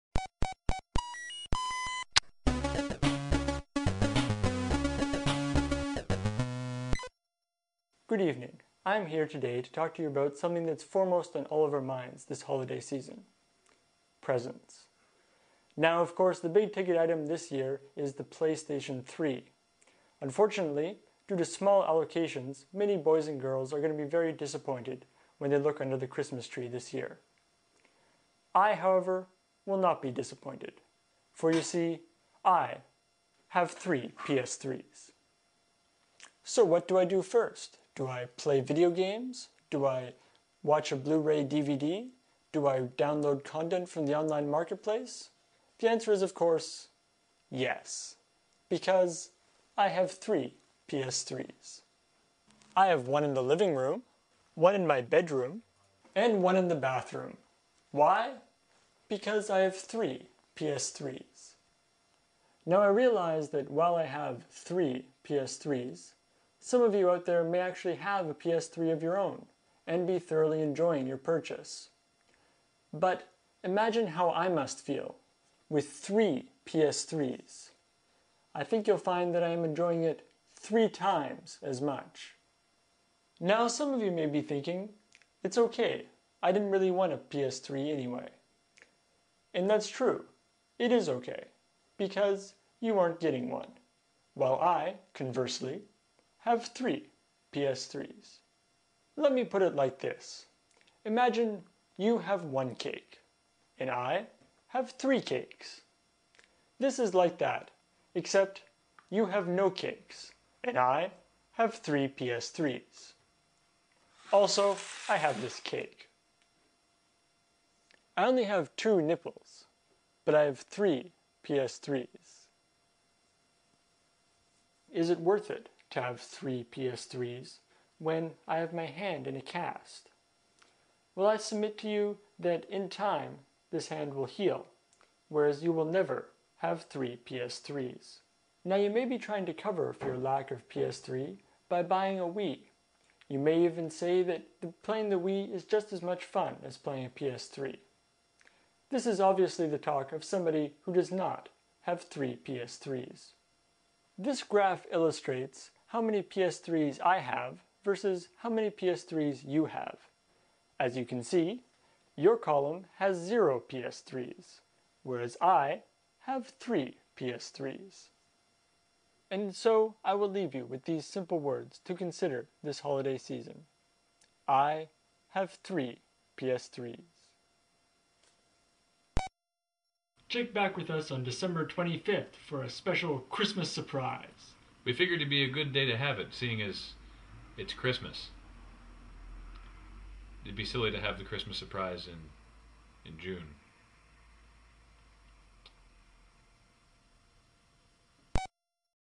Re: Three Ps3s ("casi traducción" al castellano)
He hecho esta cutre traduccion del friki que tiene 3 PS3s